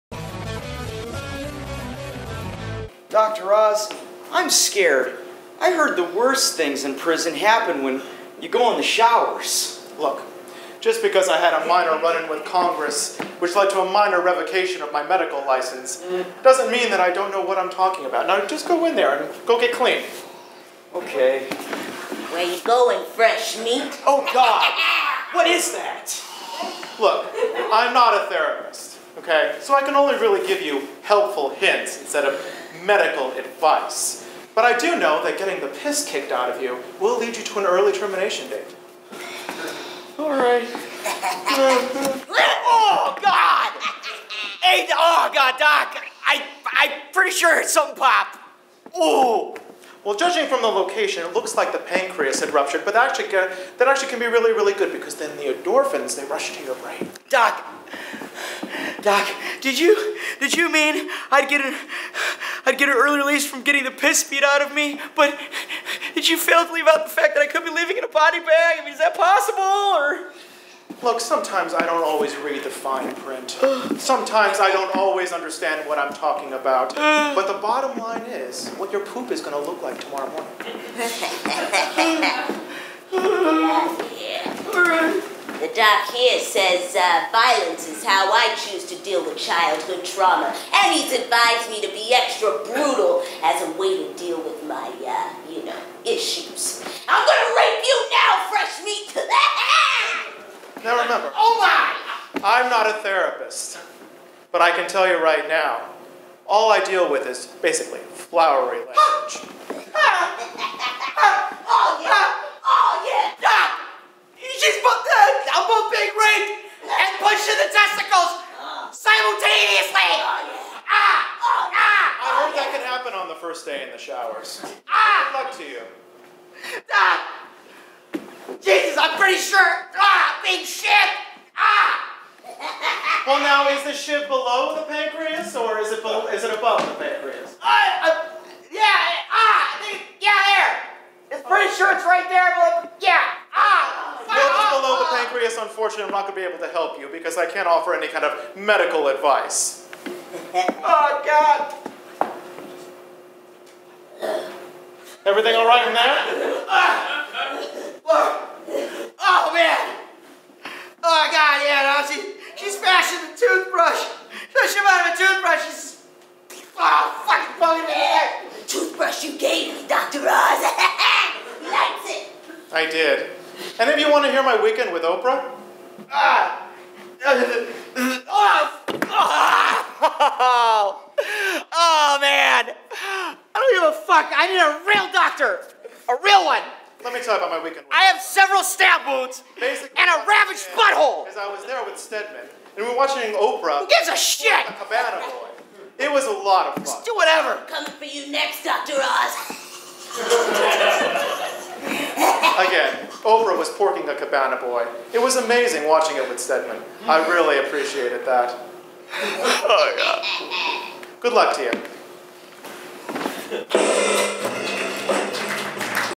Dr. Oz Goes to Jail (sketch)
Dr. Oz keeps making outrageous health claims even in the slammer.

Writer
Greg Shouse

Cast
Jimmy Guidish
Ashleigh Hairston